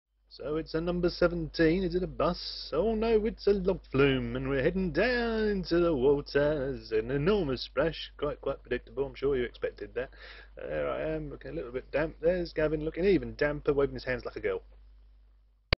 Making a splash on the Logflume (log flume?) at Alton Towers
The logflume is one of the oldest rides at the Staff's theme park. Here's some film (I say film. No film was involved) of me on it, having a fine time. The audo was dubbed on afterwards, as the camera I used didn't have sound.